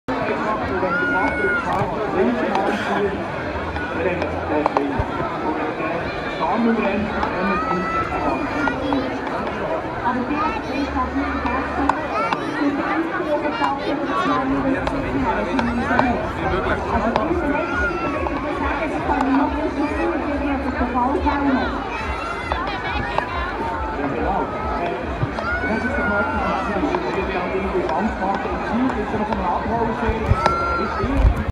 スイス発 ベルンマラソン・Grand Prix、親子マラソンからスタート！①【スイス情報.com】
スイス・ベルンのマラソン（Grand Prix）には、親子マラソンもあります。

撮影：2016年5月14日土曜日　TT